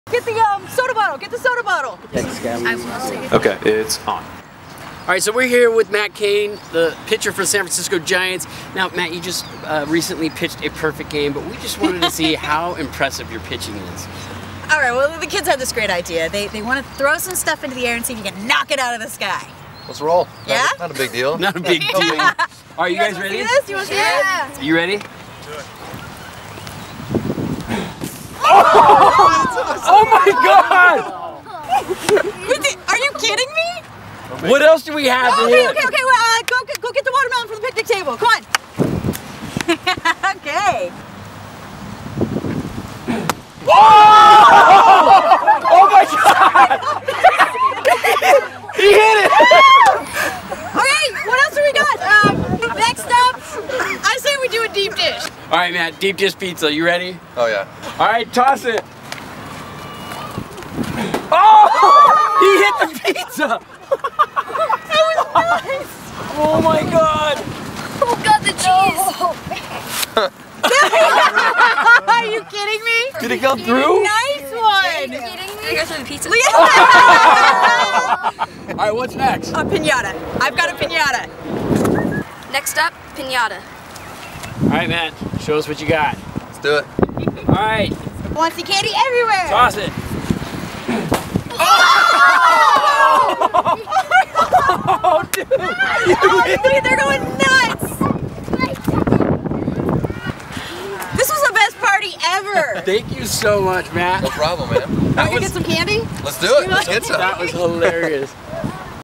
Matt Cain w/ Kari Byron & Tory Belleci from Mythbusters destroy things mid air with his fastball
Tory Belleci and Kari Byron from Mythbusters host this video of Matt Cain of the San Francisco Giants showing off his legendary throwing arm by rocketing a baseball through various objects thrown up in the air!